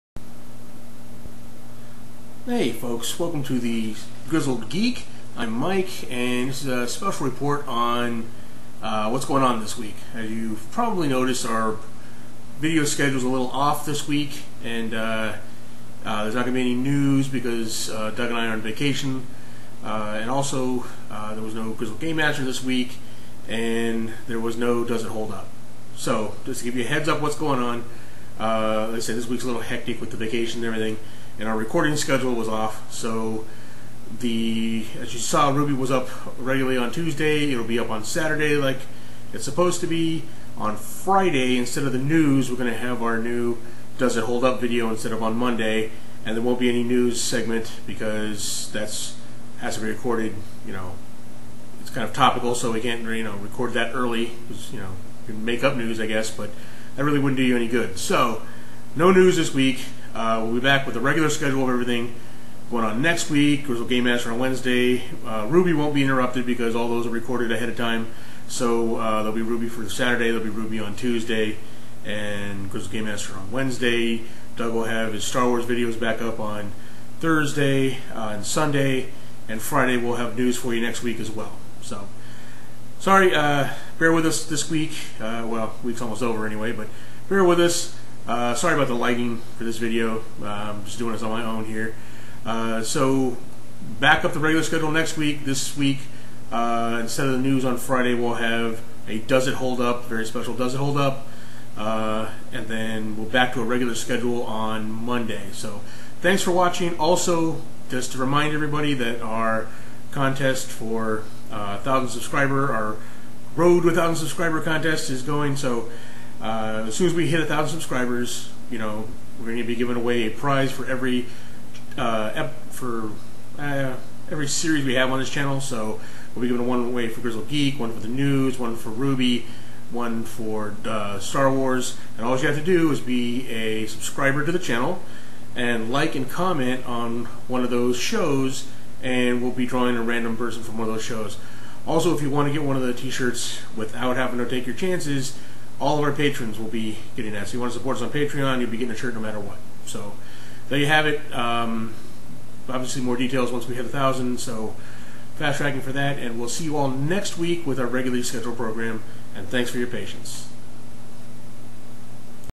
What's up with the Geeks this week?
As you probably have noticed some of our programming is missing this week. I go over the details and what to expect next week.